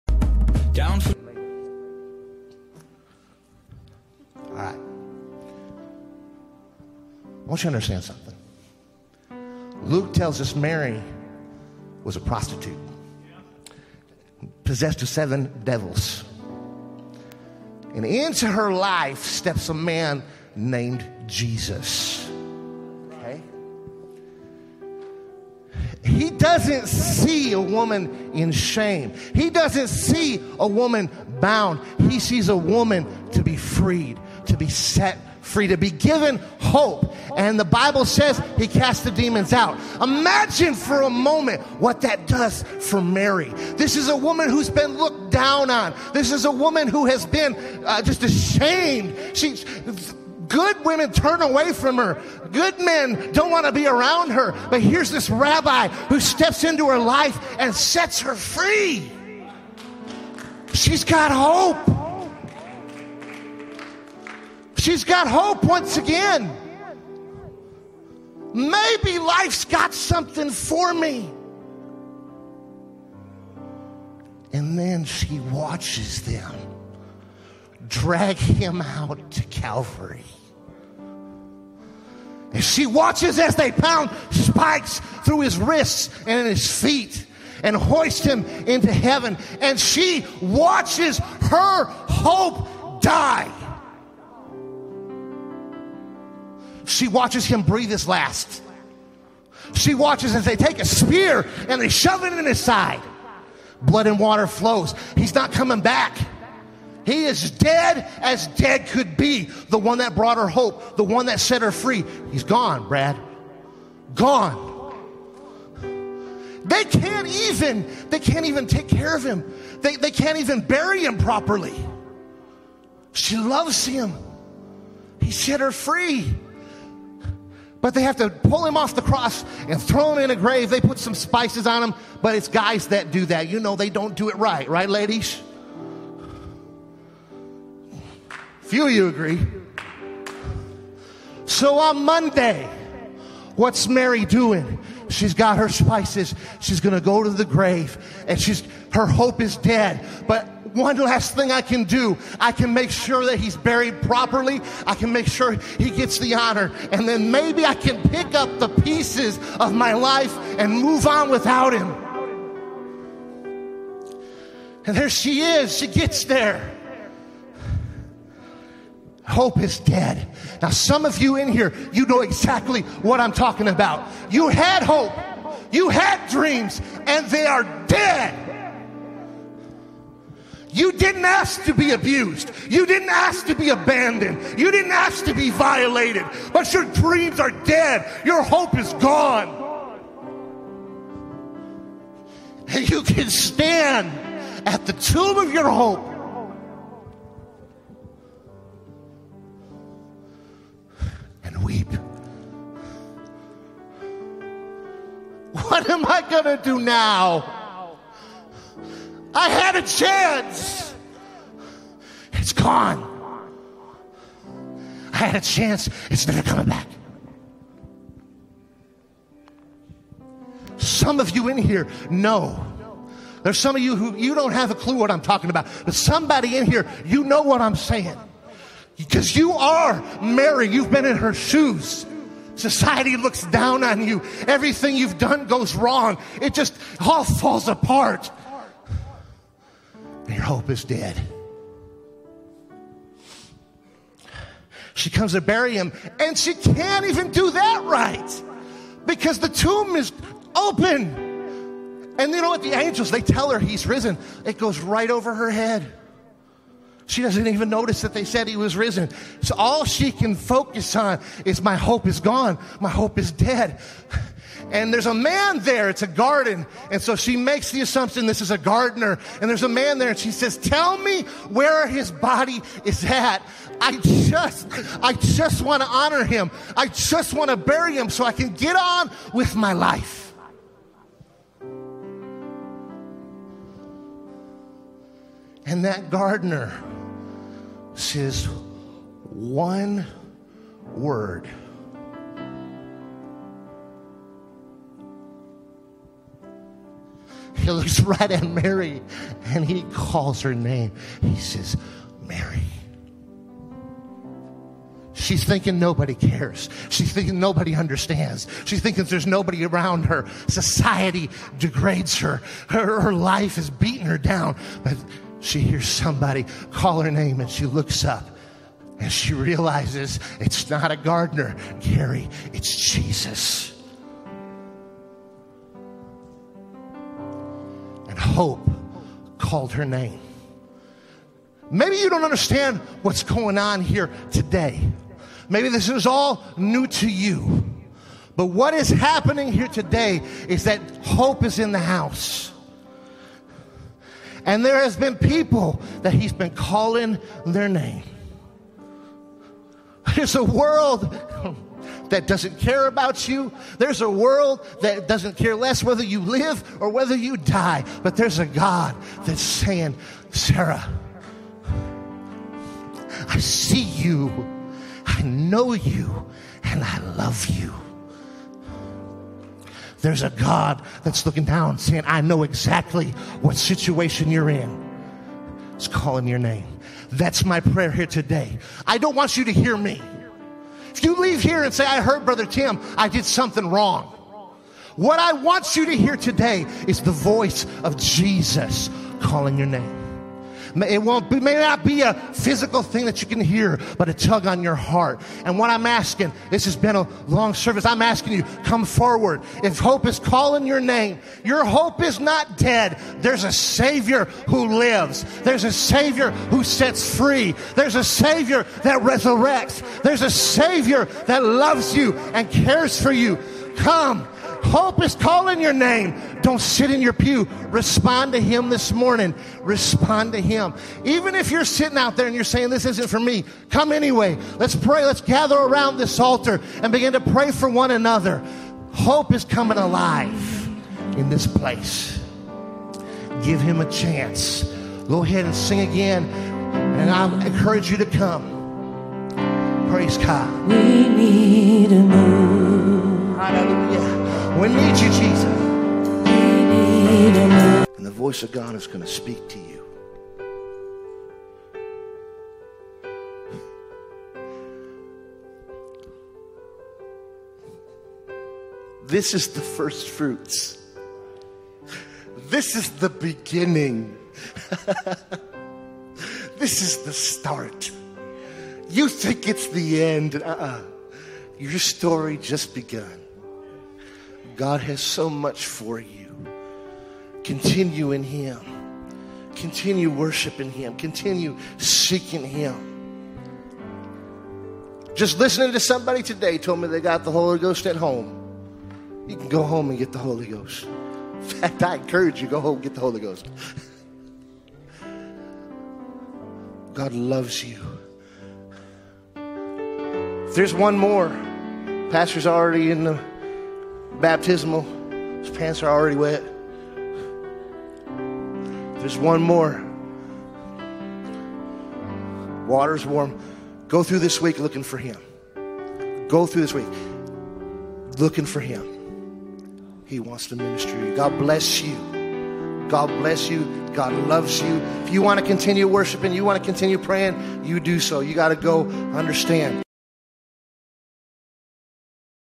When Hope Calls Your Name, Sunday, March 14, 2021
Sermon from Associate Pastor Tim Barnett.